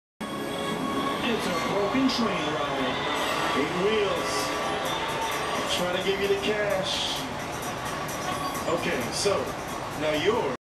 ZONK broken train ride
LMAD has an old children's ride from the scrap yard.